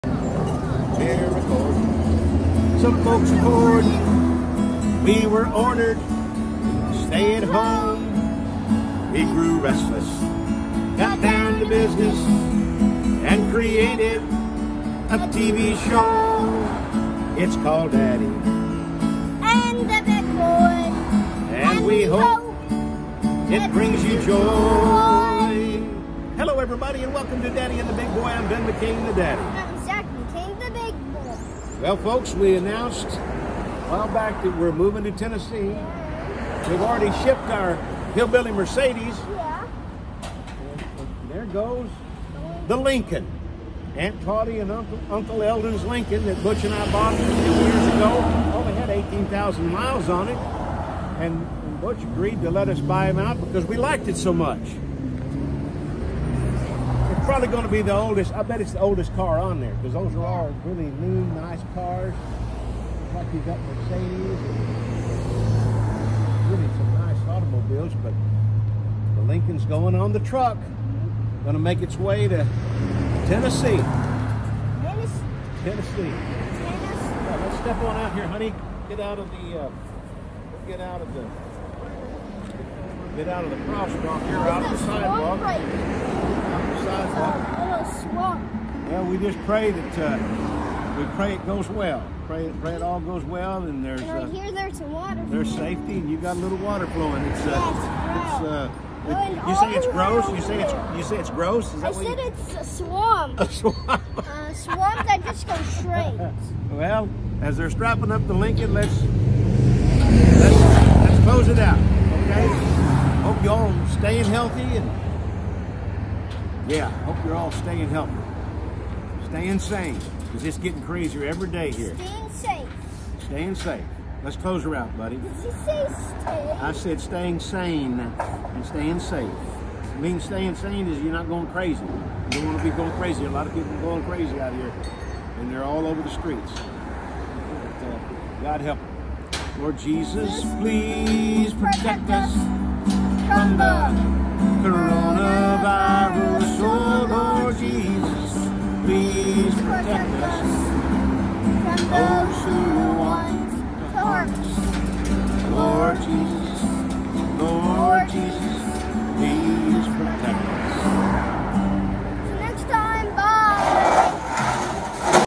Daddy and The Big Boy (Ben McCain and Zac McCain) Episode 336 Shipping the Lincoln to TN
In this episode of Daddy and The Big Boy, Ben and Zac produce a show on Washington Place in Los Angeles as their 2003 Lincoln Town Car is getting ready to be transported to Tennessee.  This show began production when Californians were ordered to stay home to try and stop the spread of the Coronavirus.  It’s a little show with lots of heart.